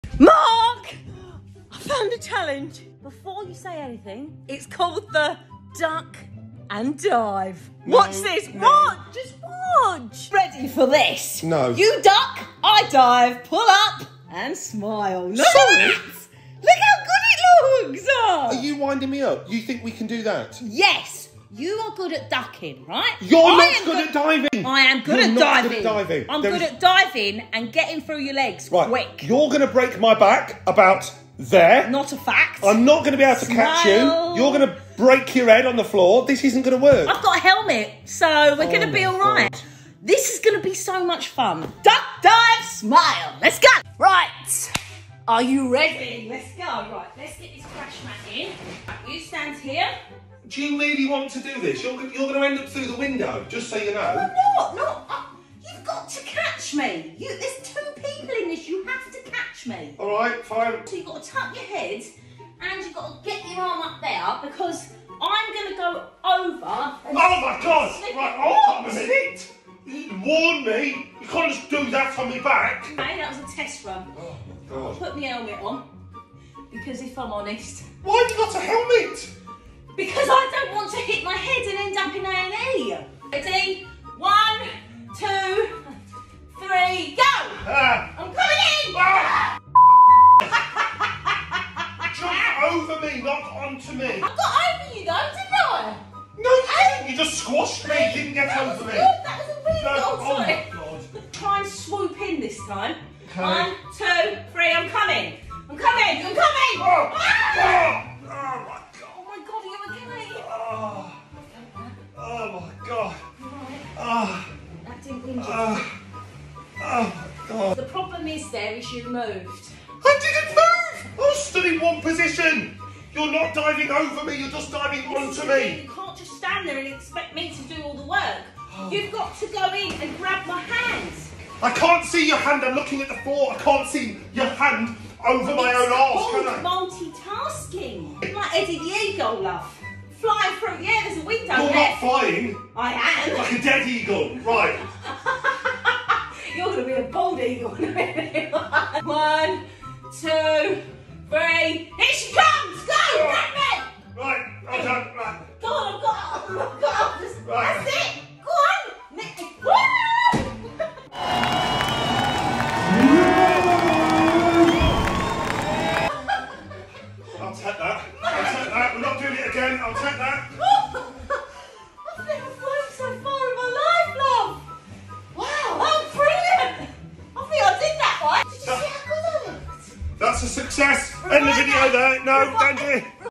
The funniest Duck & Dive challenge 🤣🦆
See what happens when Mum & Dad try the Duck and Dive Challenge 🤣 Will Mum need her crash helmet??